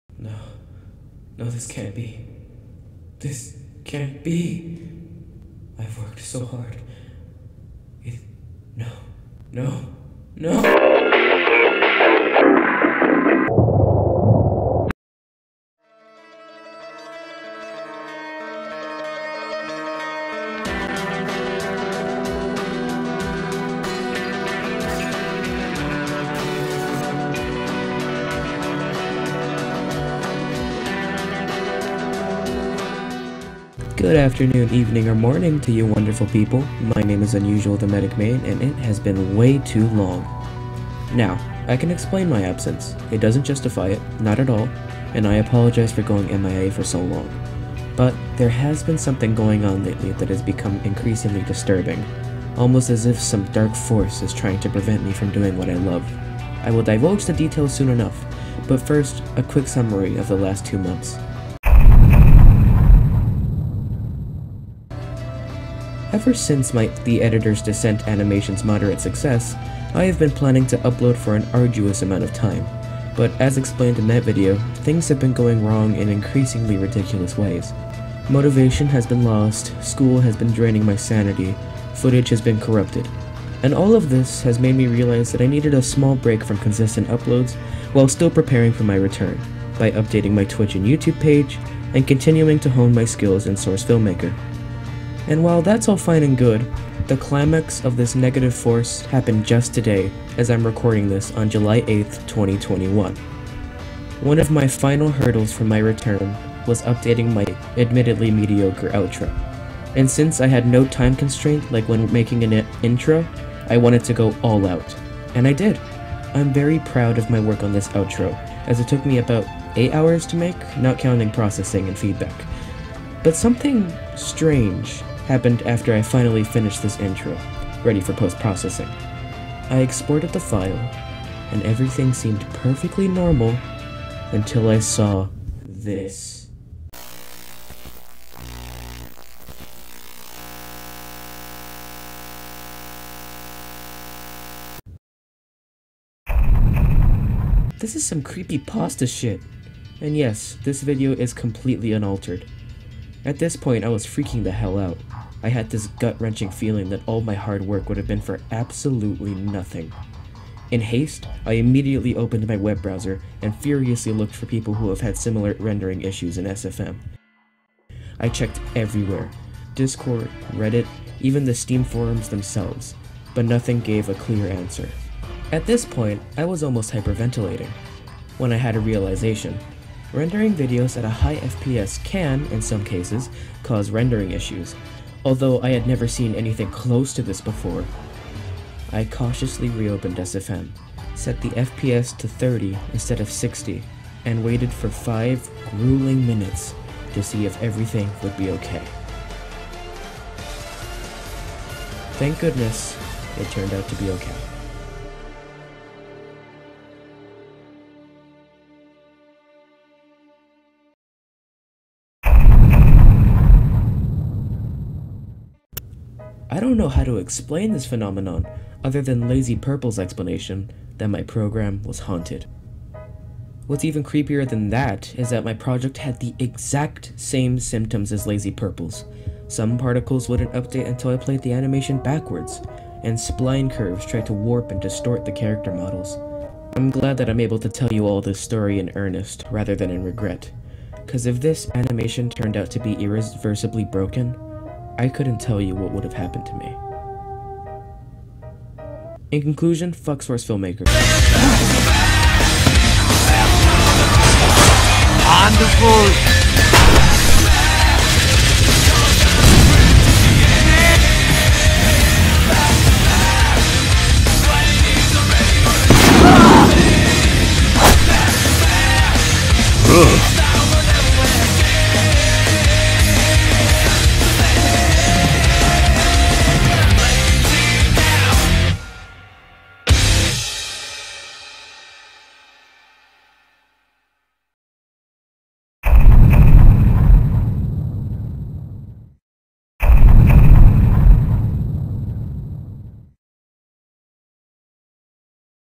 The Editor's Ascent (SFM)
Hello everyone! Sorry about my absence, but due to all of the stuff happening recently, I've gathered enough material to make a pretty entertaining story, about the CURSED OUTRO, OooOoOOoOoh.

Oh yeah! I also made a new outro, it's at the end of this video, but I'll release it later by itself, very shortly.

Timestamps.
Introduction 0:00-1:01
The Last 2 Months 1:02-2:25
What the fu 2:26-3:31
Reflecting on events past 3:32-4:09
Outro 4:10-4:56